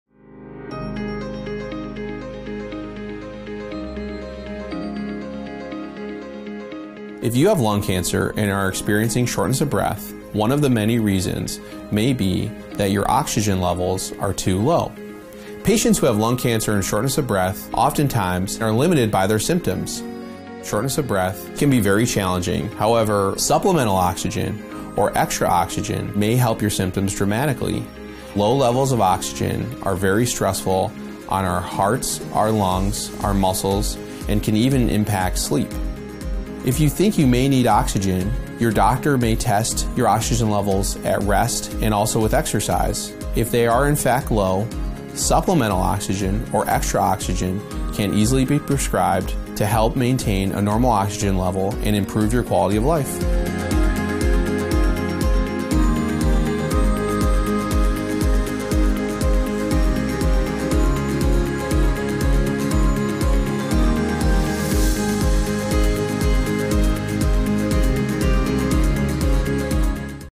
CHEST Foundation - Shortness of Breath with Lung Cancer
If you have lung cancer and are experiencing shortness of breath, it may be that your oxygen levels are too low. Oxygen reduces stress on your heart, lungs, and muscles. The CHEST Foundation has created resources on lung cancer. To learn more, visit us at lungcancer.chestnet.org.